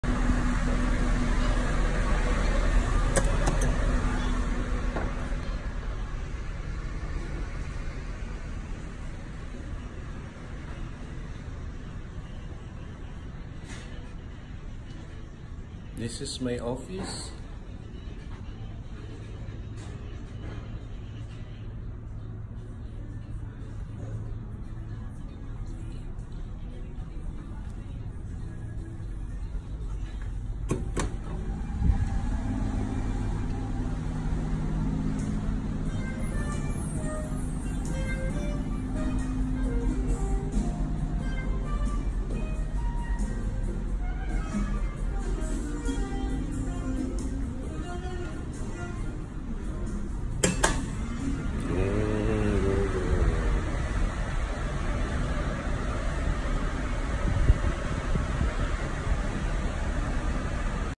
Norwegian Epic Haven
Haven area from Concierge area and private reataurant and pool